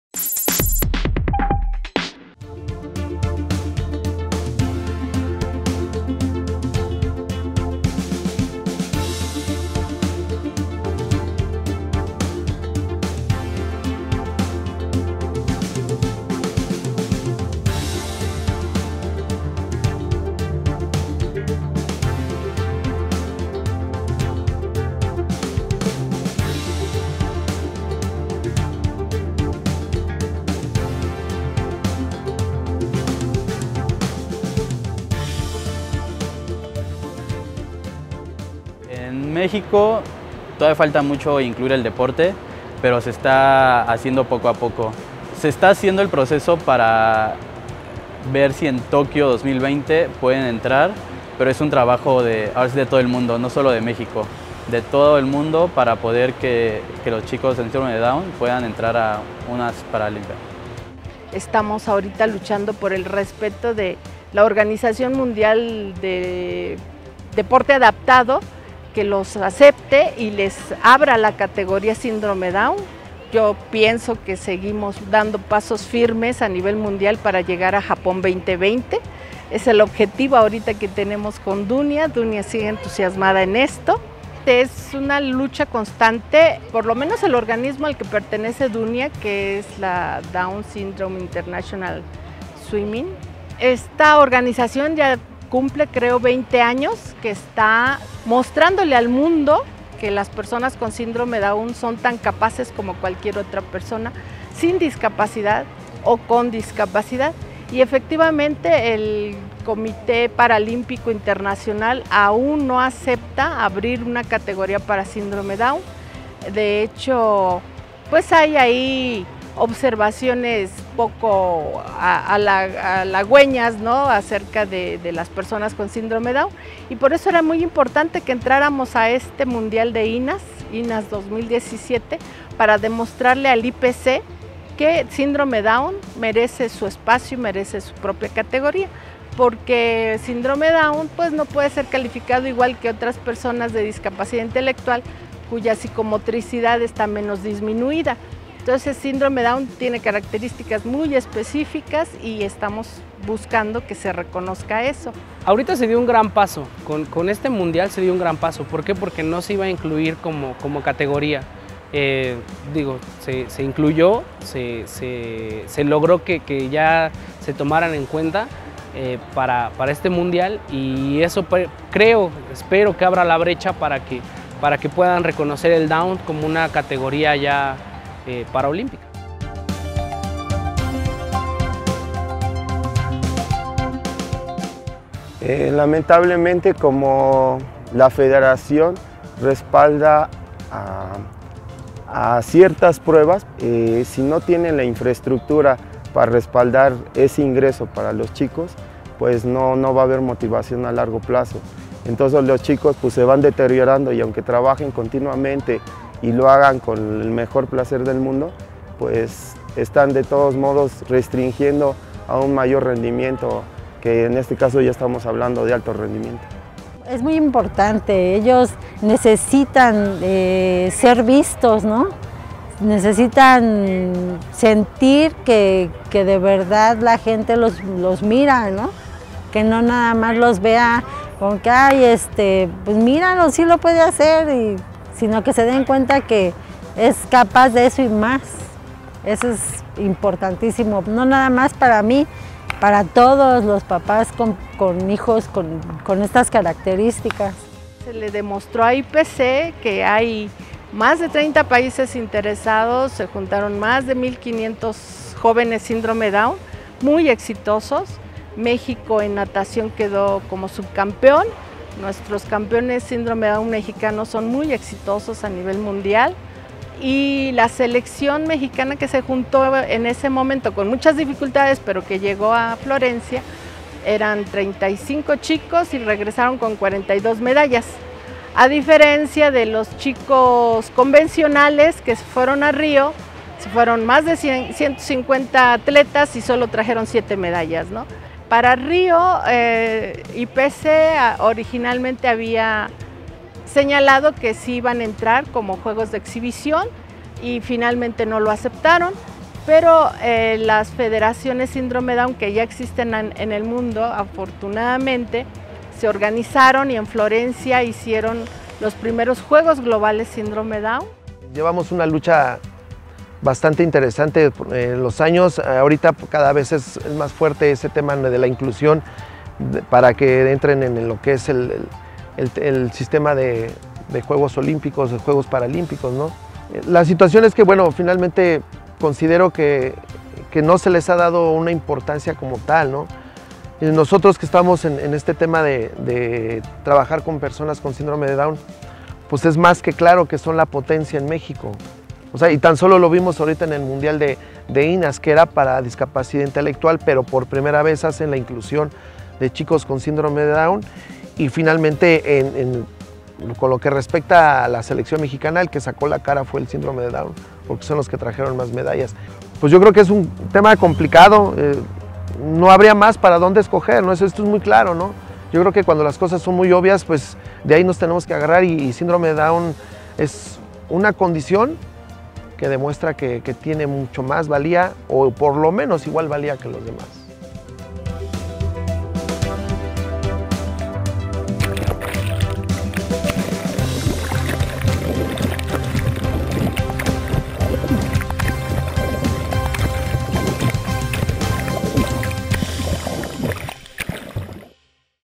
Deportistas con síndrome de down buscan lugar en Tokio 2020
Los deportistas mexicanos con síndrome de down buscan la inclusión en la justa deportiva olímpica.